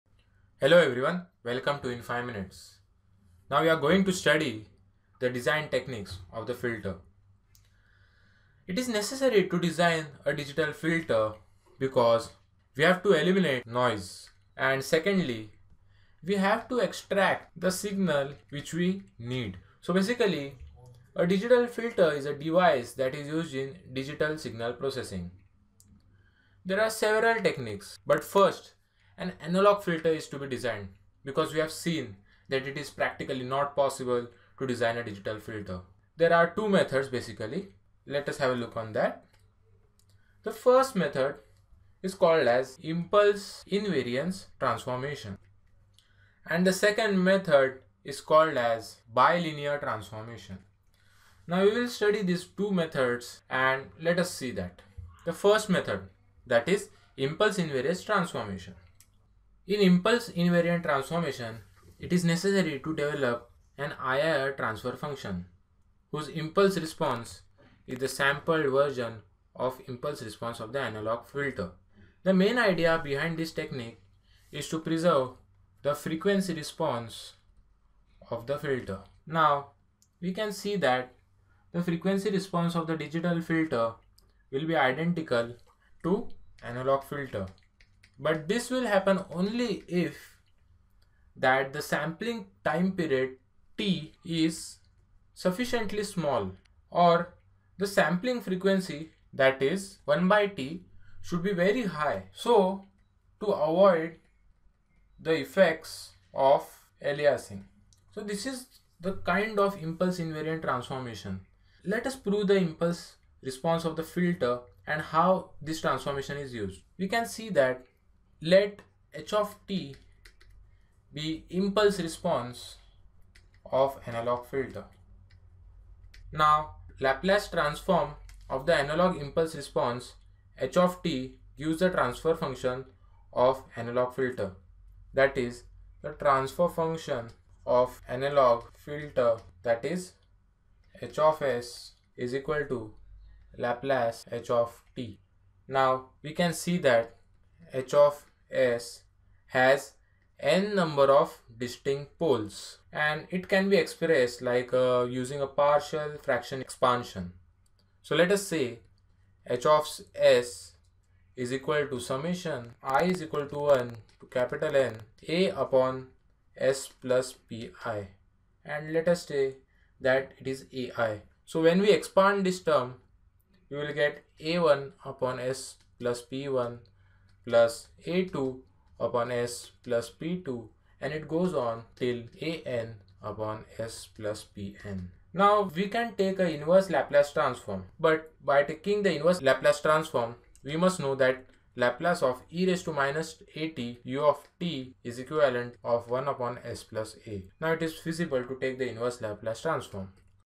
Module2_Vid_5_IIR Digital Filters_Impulse Invariance method (Part 1)
This video is an introduction for design of filters using impulse invariance and bilinear transformation method.